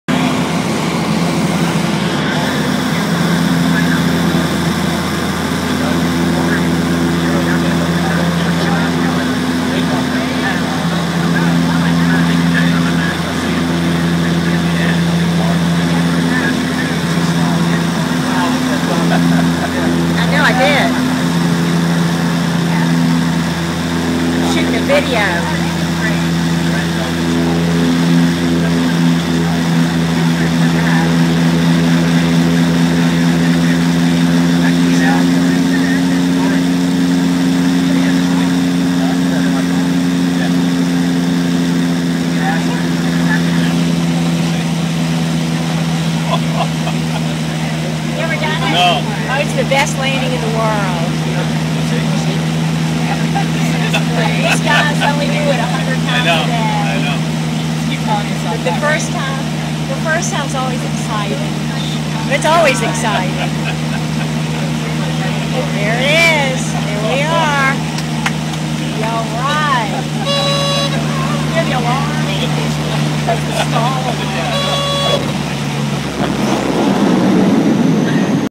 Landing in St. Barts
Steep descent on short landing strip